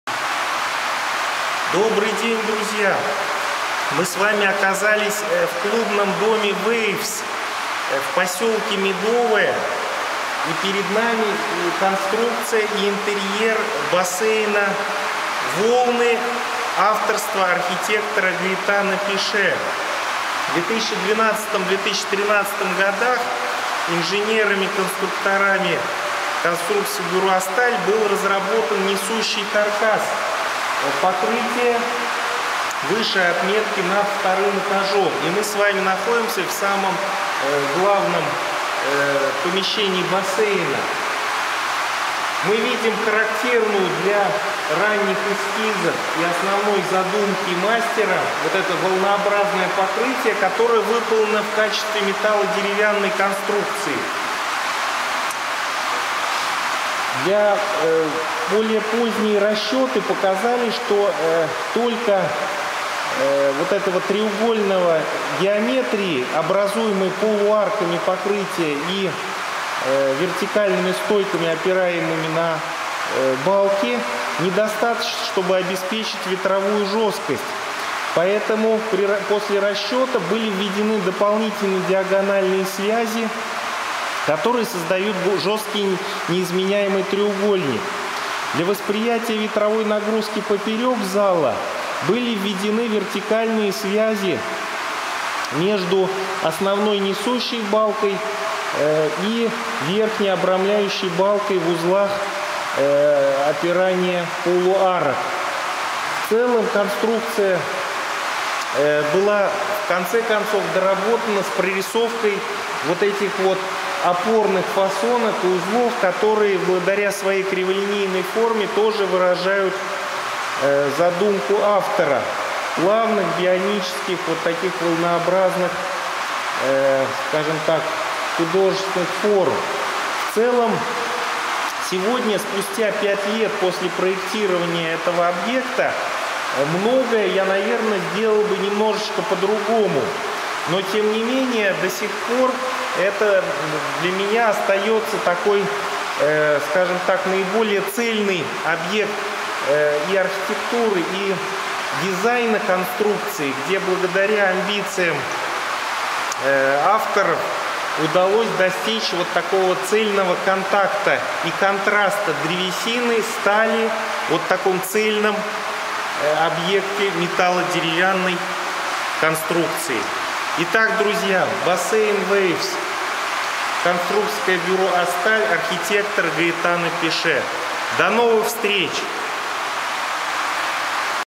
БАССЕЙН WAVES В СКОТНОМ. Часть 2
СКОТНОЕ-МЕДОВОЕ. КЛУБНЫЙ ДОМ "WAVES"(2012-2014) Архитектор Гаэтано Пеше(Gaetano Pesce) Конструкции Антон Смирнов и Александр Нароцкий. Конструкторское бюро "АСТАЛЬ"